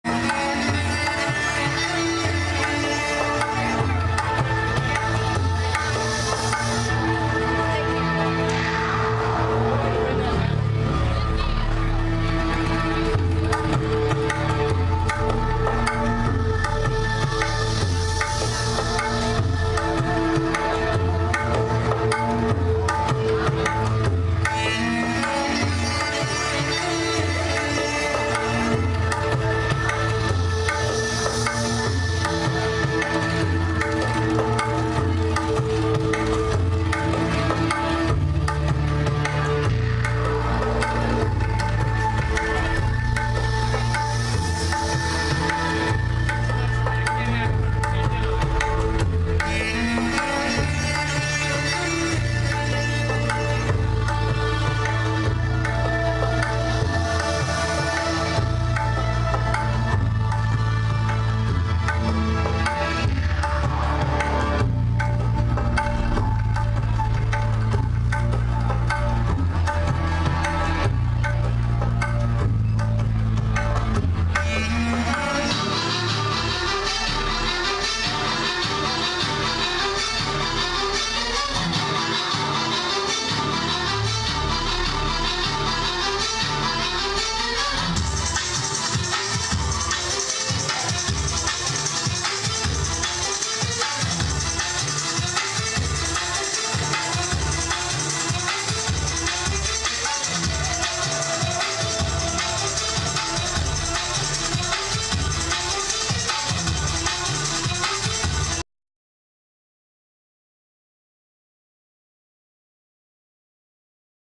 India Arabic at Viper Room, :) The Pin Up Girls
Los Angeles- Burlesque~ choreo by NY native, Vixen Romeo~ Tribal Belly Dance